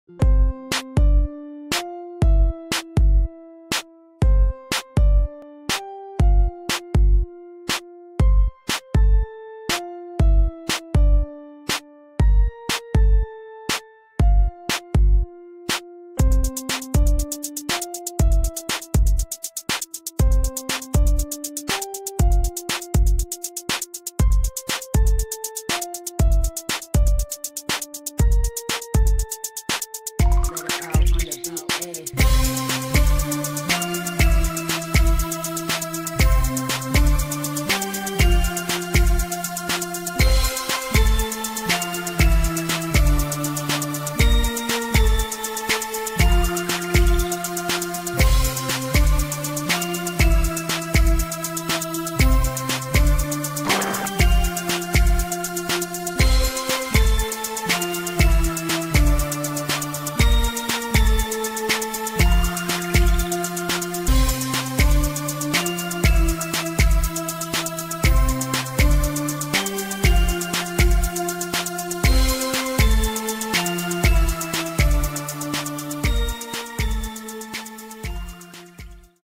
Happy Birthday, Luigi girl12!
Hello, Toonsters!

Luigi girl12 is planning a collab of her own for her birthday and this is my piece for it!

Her birthday is on February 11th.

Made using Gacha Life.

STAY SAFE AND ENJOY THE VIDEO!